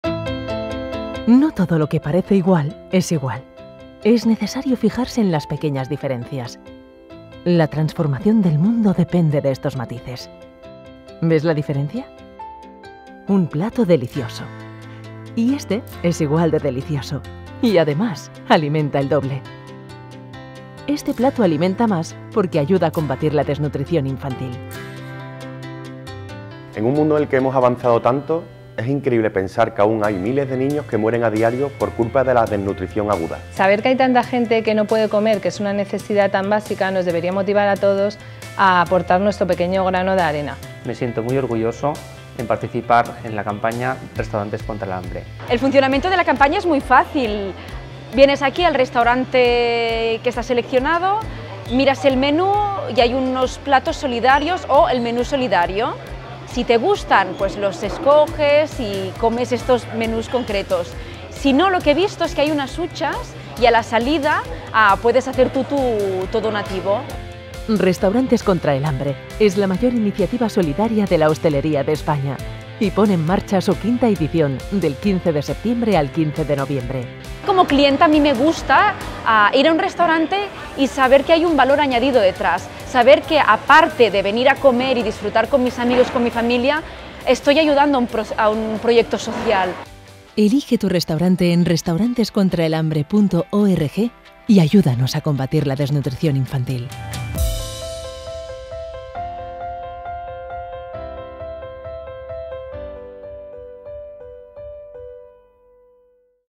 Restaurantes contra el Hambre: Platos llenos de solidaridad.
Restaurantes contra el Hambre. Una iniciativa de Acción contra el Hambre en el que los restauradores de España ofrecen a sus clientes la posibilidad de disfrutar de sus platos de manera solidaria. Busca tu restaurante solidario más cercano y de manera muy fácil, ayúdanos con nuestros proyectos.